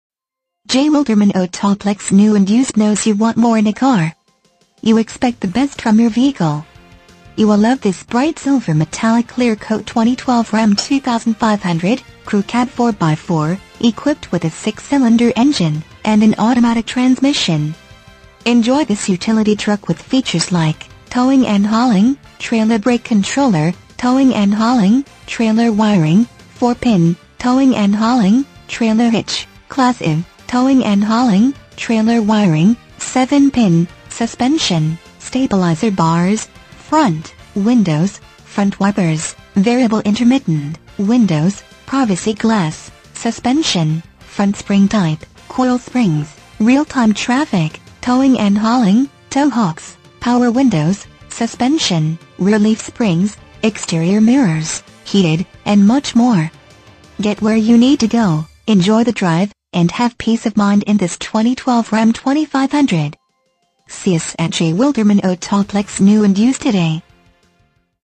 2012 RAM 2500 J Wilderman Autoplex New/Used Mt Carmel, IL 62863
RAM J Wilderman Autoplex New/Used knows you want more in a car. You have a purpose for your vehicle. Imagine driving this Bright Silver Metallic Clear Coat Ram 2500 Crew Cab 4X4, equipped with a 6 Cylinder engine and an automatic transmission.
MILES: 24,969
VIN:3C6UD5GL4CG236337

1422 West 9th St.